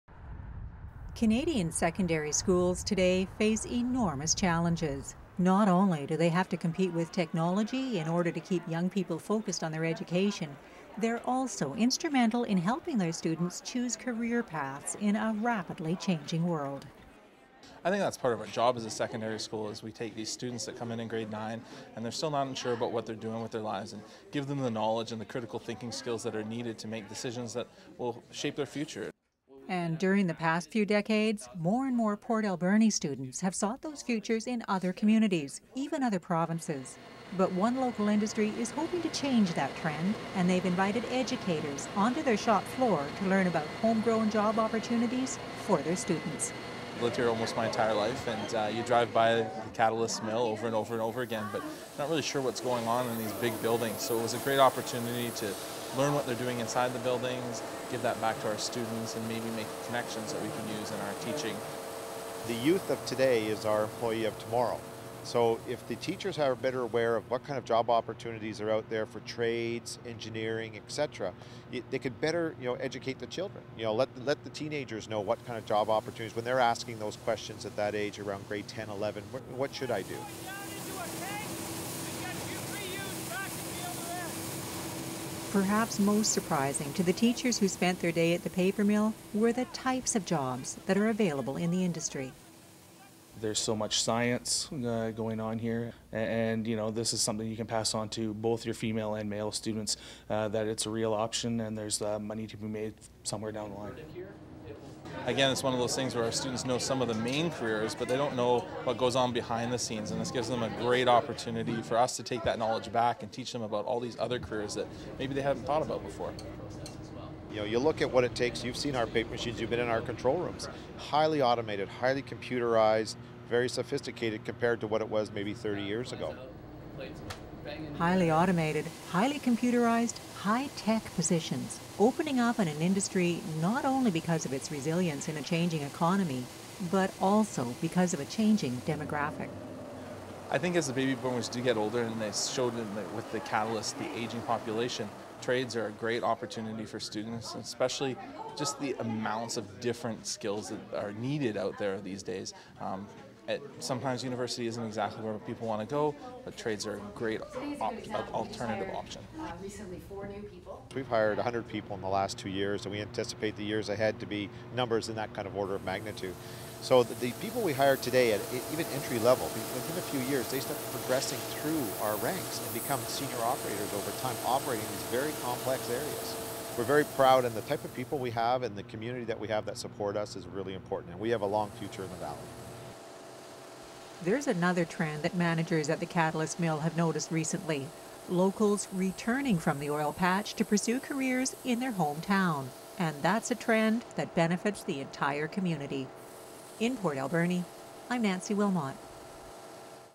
Catalyst Tour - Shaw TV Port Alberni
Trades or University?  It's a question that highschool students across North America are facing, and in Port Alberni educators are spending Professional Days learning a bit more about the options.